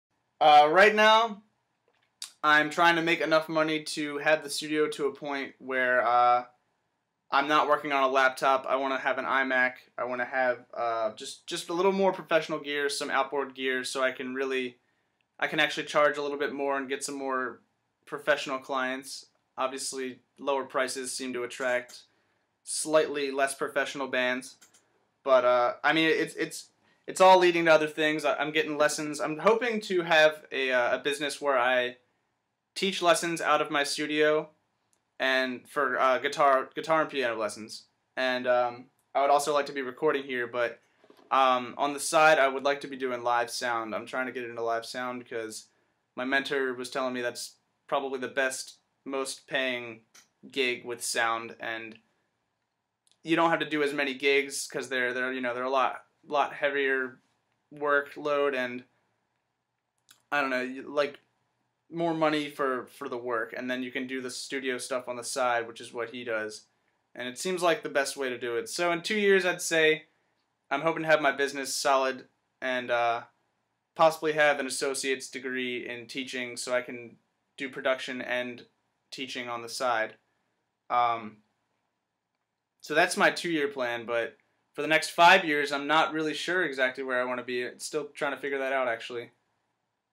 Getting Help With My Home Studio (Student Nathan Lanzino)
Right now, I'm trying to make enough money to have the studio to a point where I'm not working on a laptop. I want to have an iMac.
I want to have just a little more professional gear, some outboard gear so I can actually charge a little more and get some more professional clients. Obviously, lower prices seem to attract slightly less professional bands. But it's all leading to other things. I'm getting lessons. I'm hoping to have a business where I teach lessons out of my studio and for guitar and piano lessons. I would also like to be recording here. But on the side, I would like to be doing live sound.

I'm trying to get into live sound because my mentor was telling me that that's probably the best, most paying gig with sound and you don't have to do as many gigs because they're a lot heavier workload and I don't know, like more money for the work. Then you can do the studio stuff on the side, which is what he does. It seems like the best way to do it. So in two years I'd say, I'm hoping to have my business solid and possibly have an associate's degree in teaching so I can do production and teaching on the side. So, that's my two-year plan. But for the next five years I'm not really sure exactly where I want to be and still trying to figure that out actually.